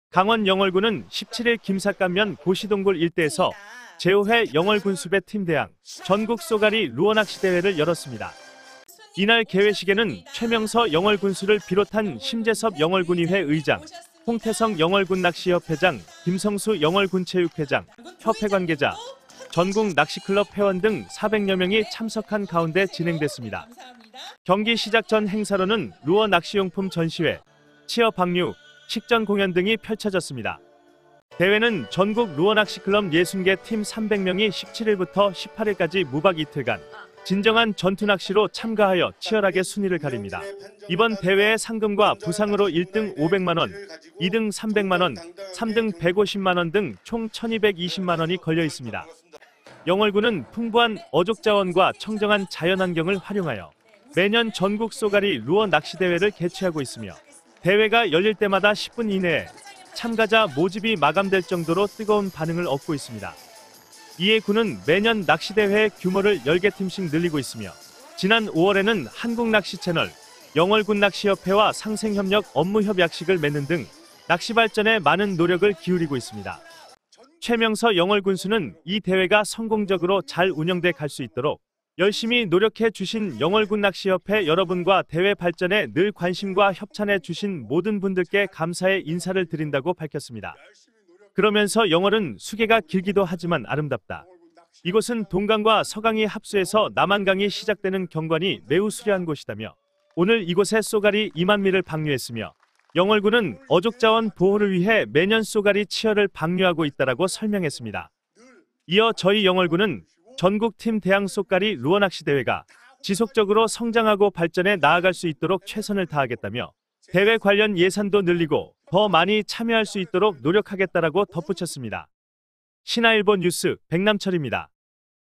영월군, 제5회 영월군수배 전국 쏘가리 낚시대회 개최 / 17일 고씨동굴 일대에서 열려 / 신아일보 (강원)
영월군, 제5회 영월군수배 전국 쏘가리 낚시대회 개최

강원 영월군은 17일 김삿갓면 고씨동굴 일대에서, 제5회 영월군수배 팀 대항 전국 쏘가리 루어낚시대회를 열었습니다.

이날 개회식에는 최명서 영월군수를 비롯한 심재섭 영월군의회 의장, 홍태성 영월군낚시협회장, 김성수 영월군체육회장, 협회관계자, 전국 낚시클럽 회원 등 400여명이 참석한 가운데 진행됐습니다.

경기 시작 전 행사로는  루어 낚시용품 전시회, 치어 방류, 식전공연 등이 펼쳐졌습니다.

대회는 전국 루어낚시클럽 60개 팀 300명이 17일부터 18일까지 무박 이틀간 진정한 전투낚시로 참가하여 치열하게 순위를 가립니다.이번 대회에 상금과 부상으로 1등 500만원, 2등 300만원, 3등 150만원 등 총 1,220만원이 걸려있습니다.

영월군은 풍부한 어족자원과 청정한 자연환경을 활용하여 매년 전국 쏘가리 루어 낚시대회를 개최하고 있으며, 대회가 열릴 때마다 10분 이내에, 참가자 모집이 마감될 정도로 뜨거운 반응을 얻고 있습니다.

이에 군은 매년 낚시대회의 규모를 10개팀씩 늘리고 있으며 지난 5월에는 한국낚시채널, 영월군낚시협회와 상생협력 업무협약식을 맺는 등 낚시발전에 많은 노력을 기울이고 있습니다.

최명서 영월군수는 "이 대회가 성공적으로 잘 운영돼 갈 수 있도록 열심히 노력해 주신 영월군낚시협회 여러분과 대회 발전에 늘 관심과 협찬해 주신 모든 분들께 감사의 인사를 드린다"고 밝혔습니다.

그러면서 "영월은 수계가 길기도 하지만 아름답다.이곳은 동강과 서강이 합수해서 남한강이 시작되는 경관이 매우 수려한 곳이다"며 "오늘 이곳에 쏘가리 2만미를 방류했으며 영월군은 어족자원 보호를 위해 매년 쏘가리 치어를 방류하고 있다"라고 설명했습니다.

이어 "저희 영월군은, 전국 팀 대항 쏘가리 루어낚시대회가 지속적으로 성장하고 발전해 나아갈 수 있도록 최선을 다하겠다"며 "대회 관련 예산도 늘리고, 더 많이 참여할 수 있도록 노력하겠다"라고 덧붙였습니다.

신아일보 뉴스, 백남철입니다.